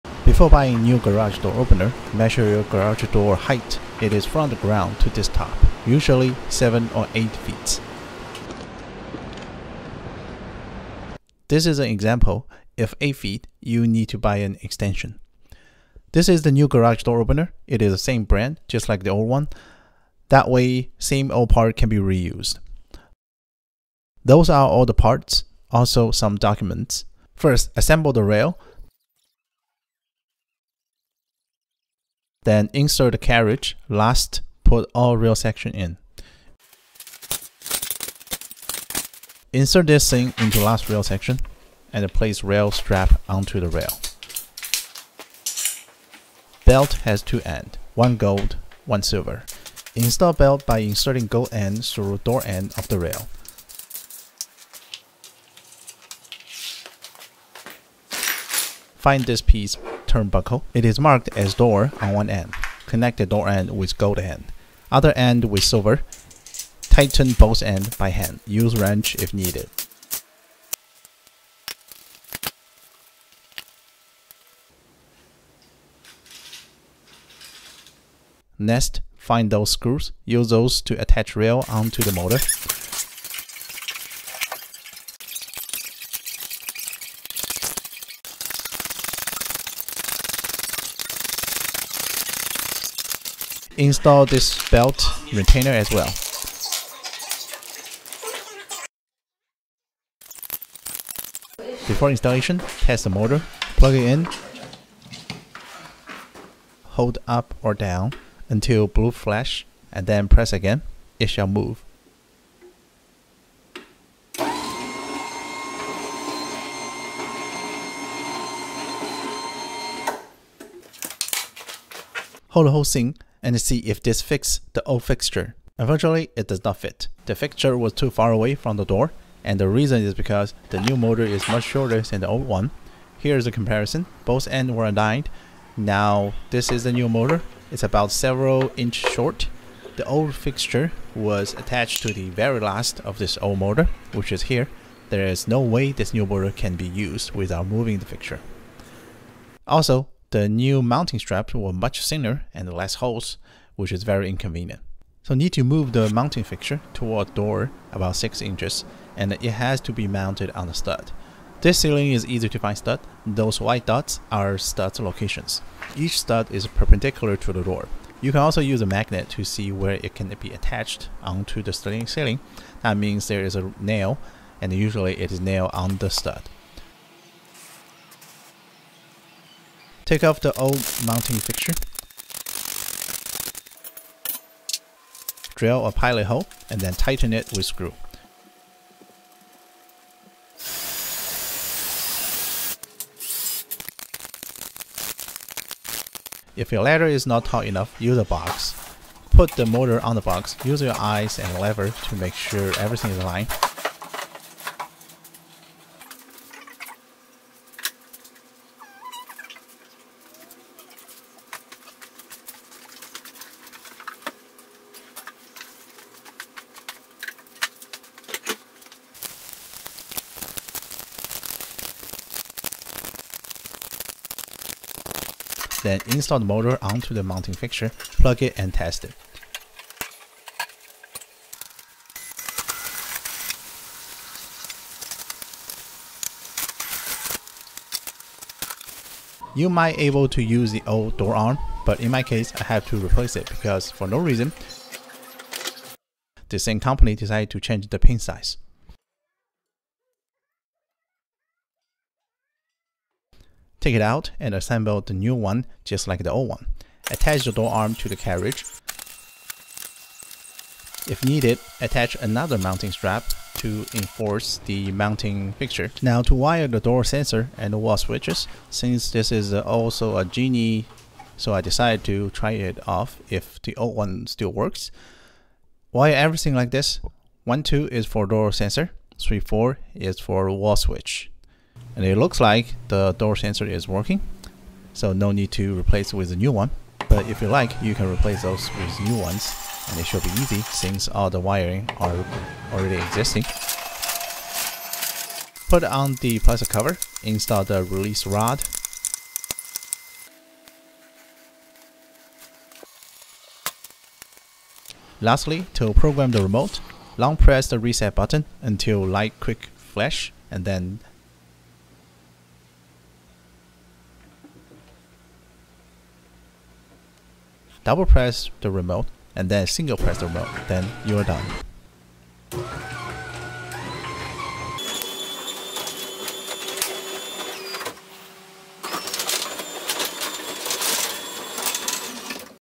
How to install Belt Drive Garage Door Opener - Reuse some old parts
Genie Smart 1 HPc Belt Drive Garage Door Opener with integrated LED lighting
Model # 39765Y/3155L-TV